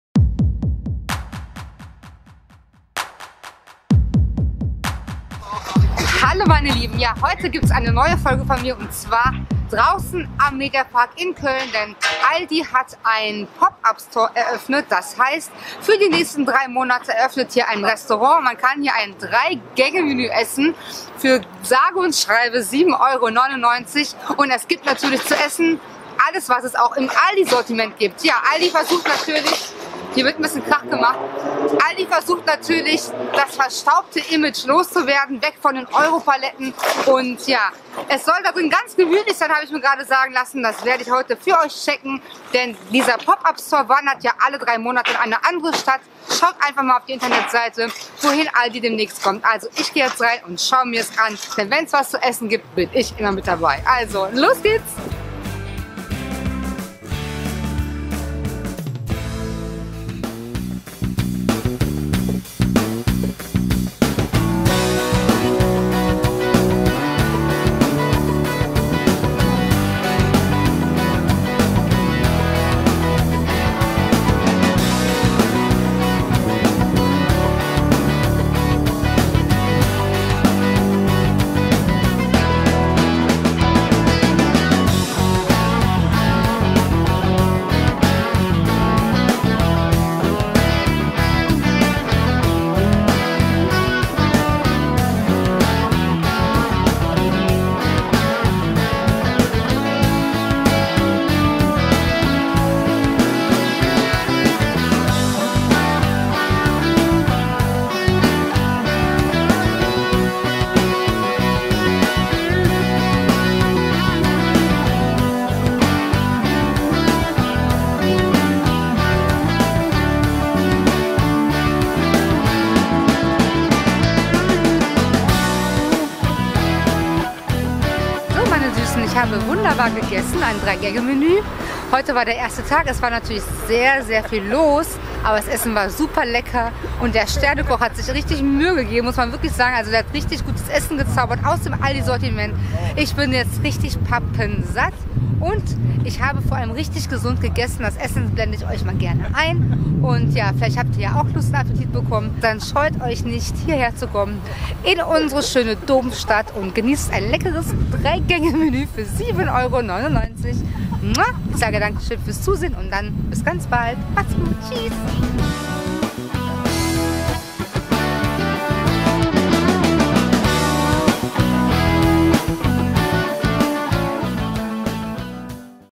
Aldi Bistro Pop Up Store Cologne | Linda Daskewitz
Der Handelsriese ALDI goes Bistro! Ich war dort vor Ort und zeige Euch das absolut coole Aldi PopUp Store ! Beeilt Euch, es bleibt nur 3 Monate in Köln, bevor es weiterwandert! Und lecker war's auch ! Freu mich, wenn es Euch gefällt! Lasst mir ein Däumchen hoch da, wenn Ihr mehr solcher Videos sehen möchtet. Eure Linda😘😘😘

*Mein Schmuck
*Meine Lederjacke